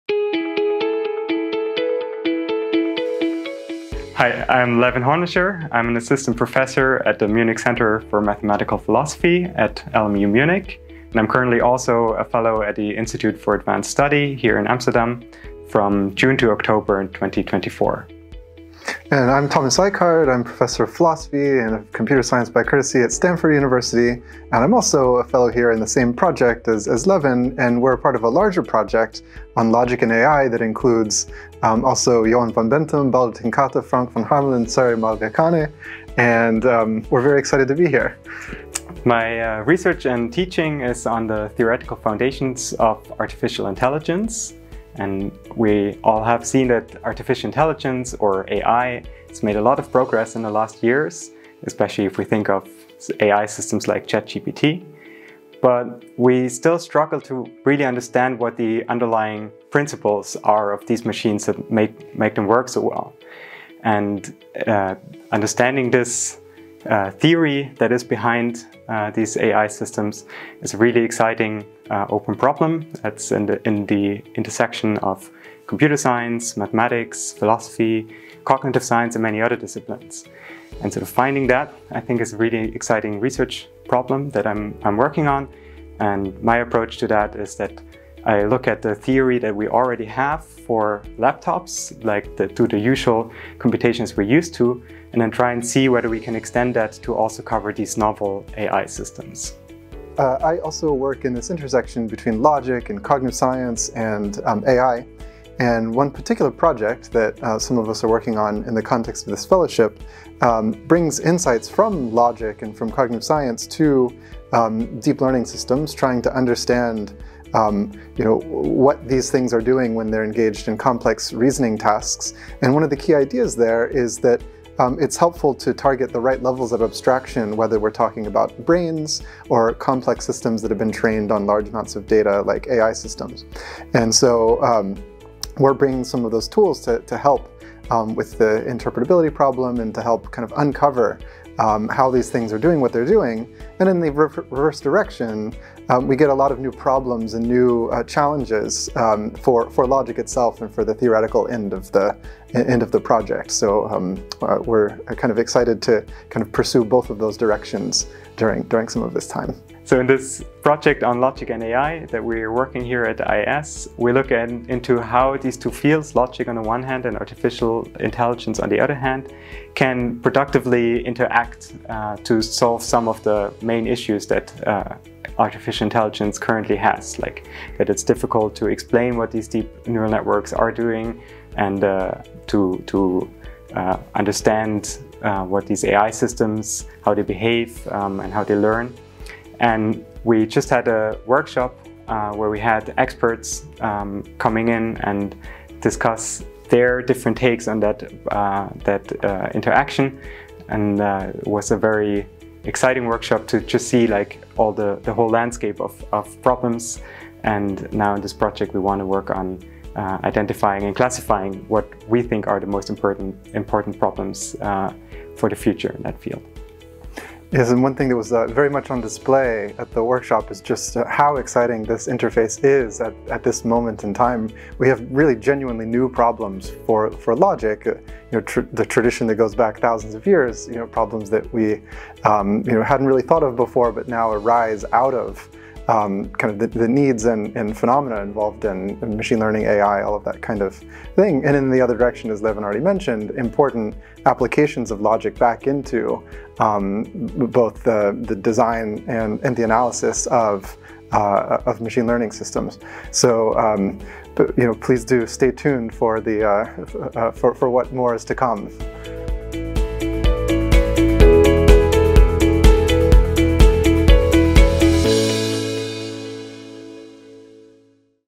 IAS Fellows Levin Hornischer & Thomas Icard on identify interactions between logic and AI
Levin Hornischer and Thomas Icard started their fellowship in June 2024. In this video, they will explain their research in short and what their fellowship at IAS will be focussed on.